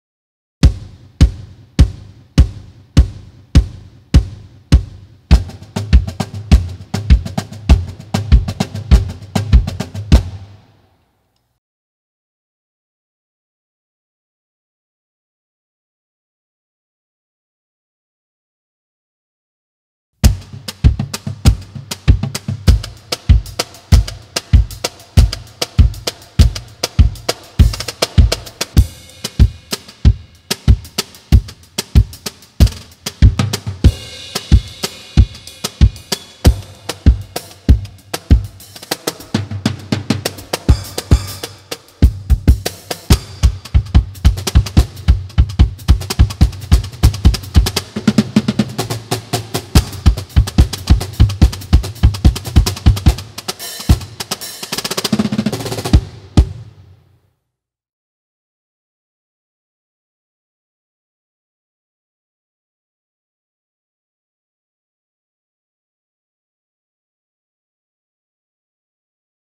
GIGBOX by Cajon Percussion
Apresentamos o GIGBOX, a solução para bateristas e percussionistas que buscam mais praticidade com qualidade, sonoridade,  timbre, design moderno e ergonomia elevados a um outro nível.
GIGBOX, a solução para sua GIG!